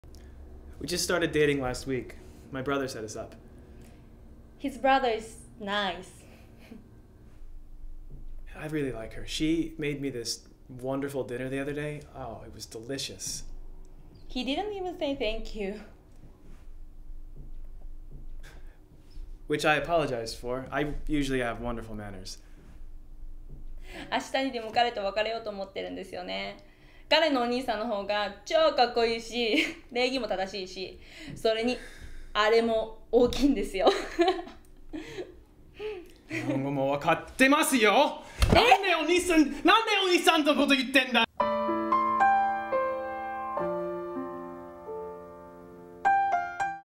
Ad Council Parody Commercial #2
Parody of Ad Council Commercial starring Bryant Sullivan and Lisa Itabashi.